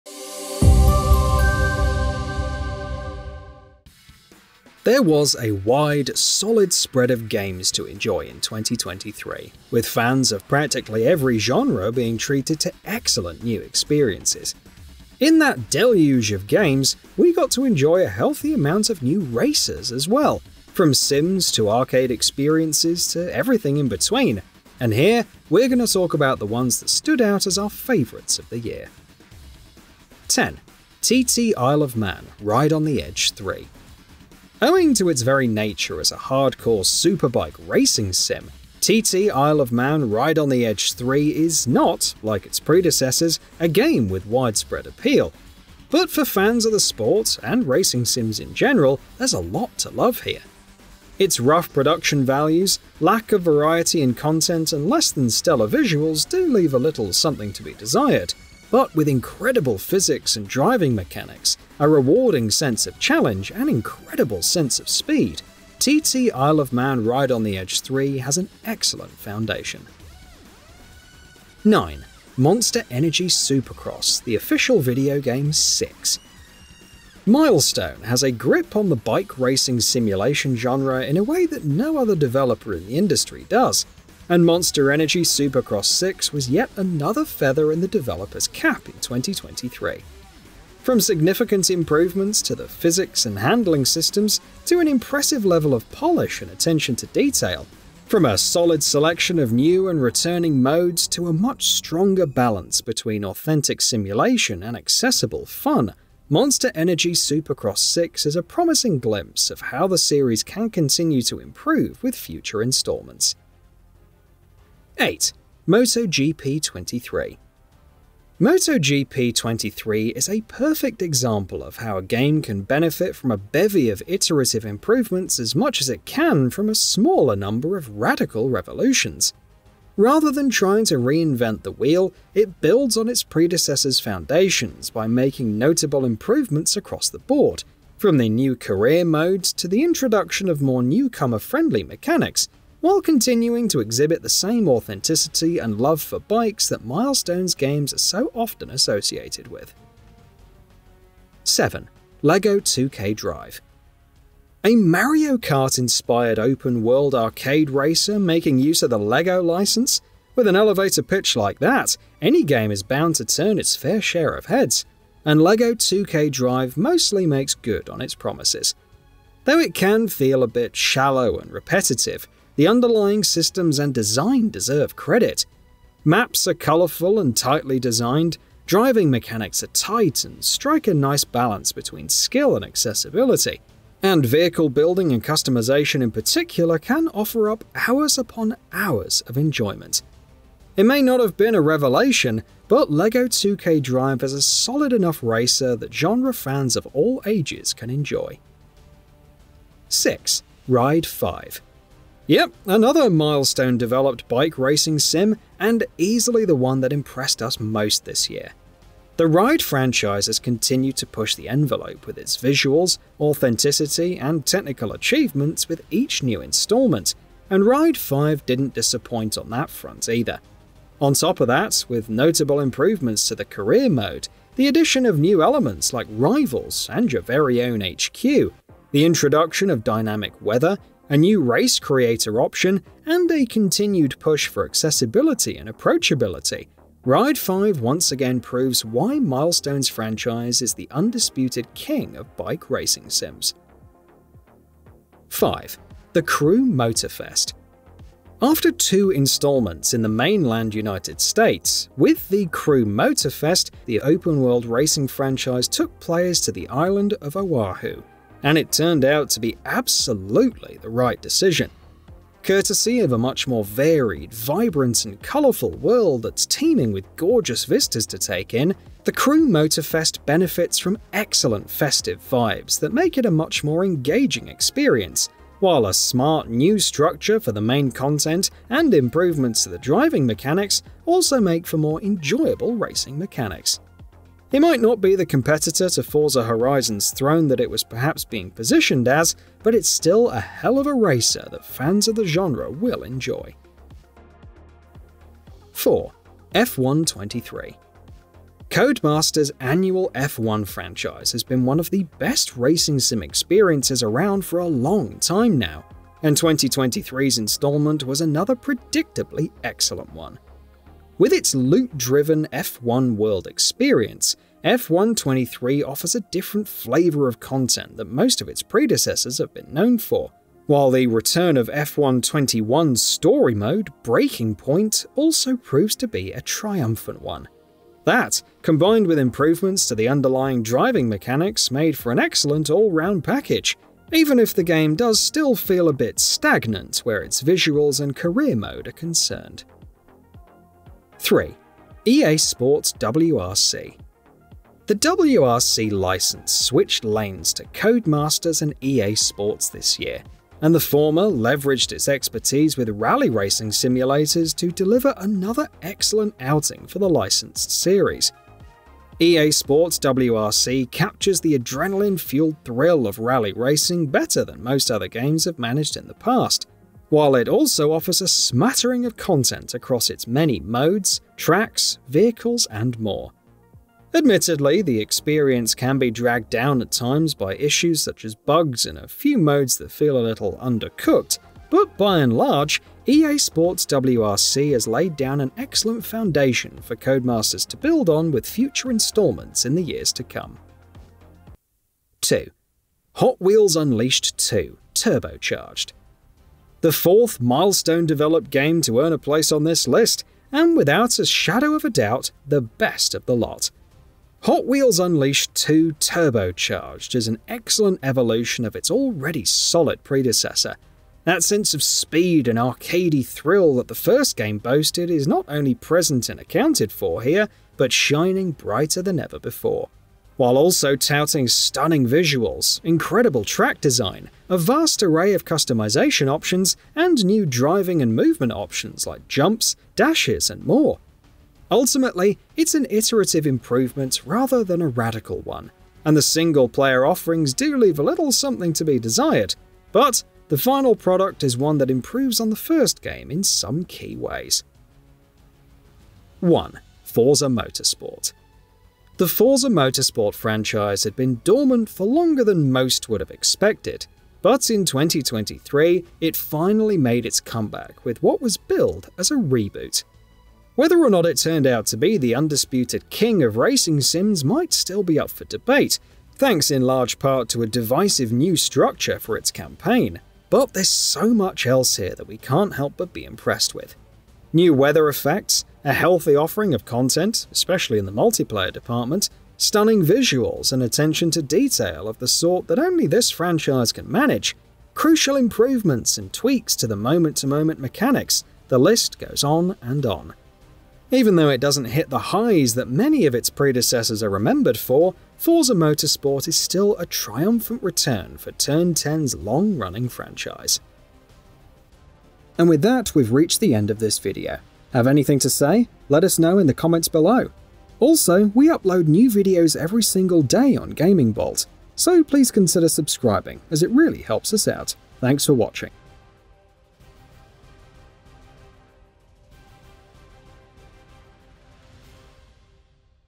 Top 10 Racing Games of 2023 You DEFINITELY Need To Play
Racing games, like so many other genres, enjoyed a pretty solid 12 months in 2023. From big developers to small, from arcadey experiences to authentic simulators, from bikes to supercars to Lego bricks, from open worlds to stage-based racers, there was a healthy and impressively varied selection of new games to play for fans of the racing genre throughout the course of the year.

Perhaps we didn’t get a single game that was able to stand out as one of the genre’s best offerings ever the way so many other genres did in 2023, but collectively, the steady stream of racers that we played throughout the year delivered on several fronts. Here, we’re going to recognize a few of those games, and talk about the ones that stand out as our favorites.